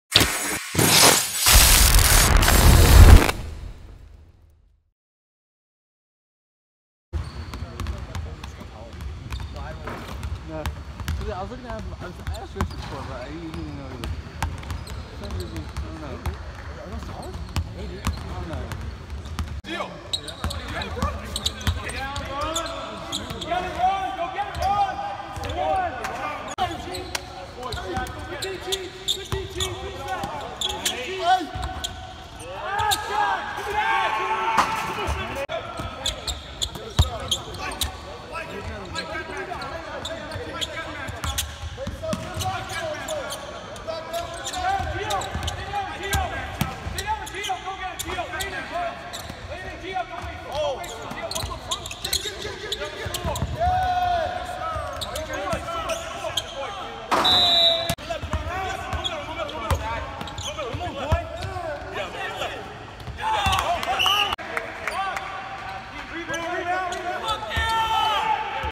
Rutgers Scarlet Knights Basketball - Practice No.1 Highlights / 1on1's
Rutgers Basketball opened practice today for the first time this season. Here are a couple of highlights from the one on ones from earlier today.